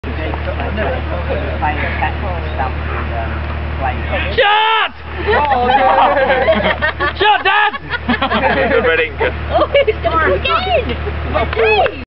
Castor & Ailsworth Cricket Club's New Professional!!!
The most recent footage of Castor's all new professional, allegedly on £50k per run!!